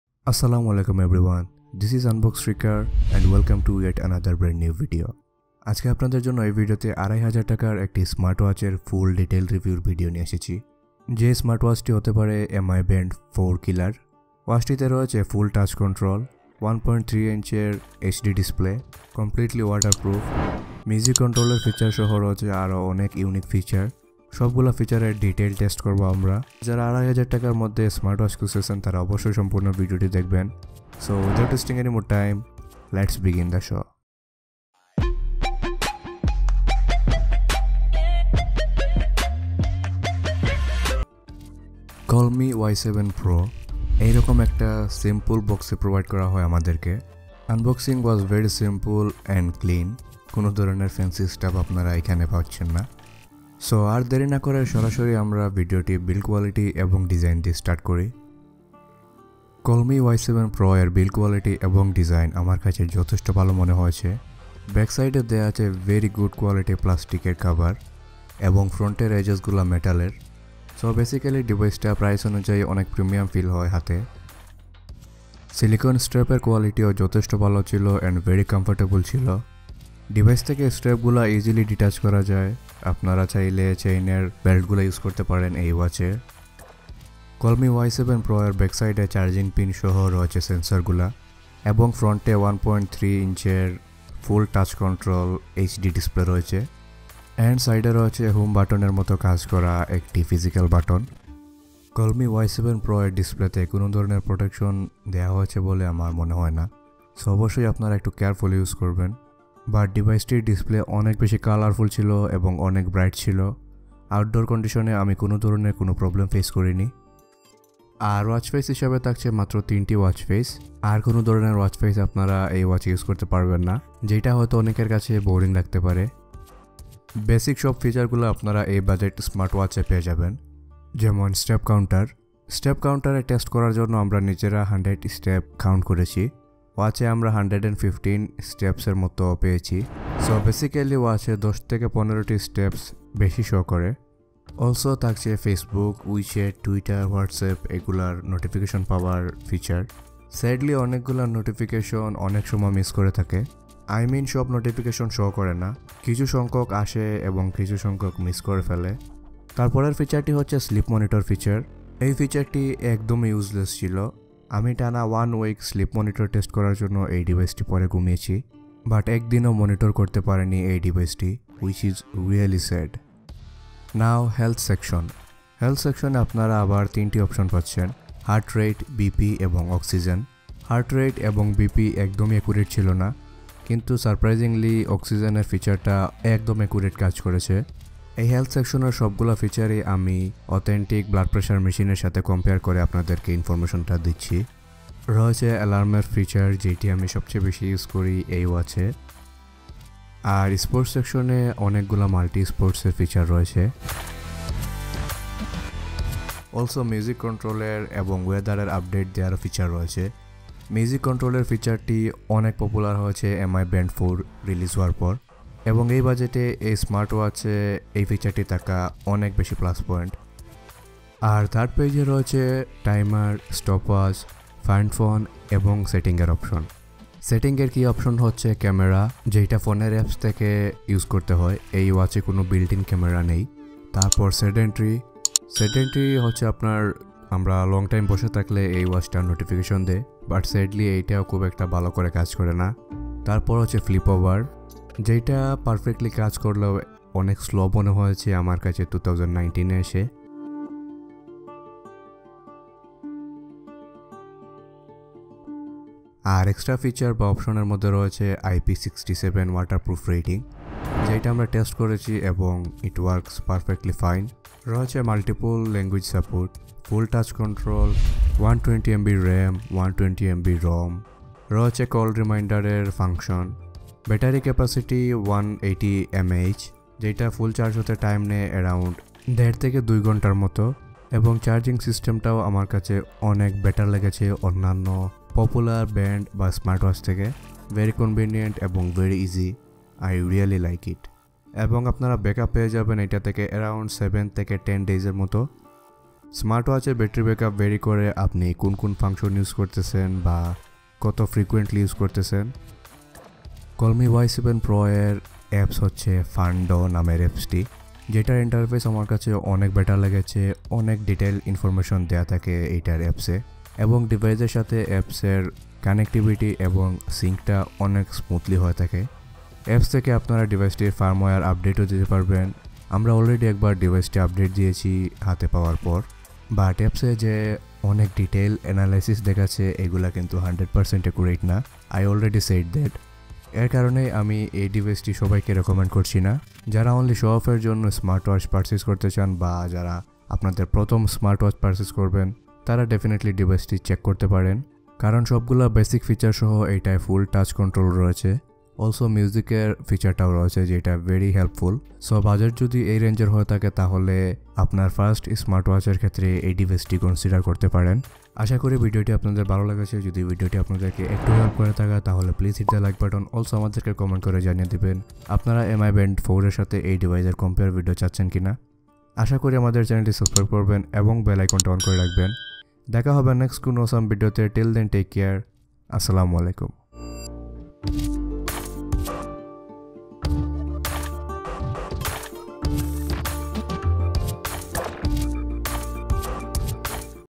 Colmi Y7 Pro Evertything You Need In Budget | Mi Band 4 Killer?? | Bangla
Hi,This Is Unbox Freaker.Today We Will Review One Of The Budget Smart Watch Called Colmi Y7 Pro.Colmi Y7 Pro Smartwatch Has Full Touch Control,Ip67 Waterproof Rating,Sports Feature And Many More.Lets Check Out How It Is.And Check Is It Actually A Mi Band Killer???

📷Gear I Use:
1.Camera (USA)
2.Lens (USA)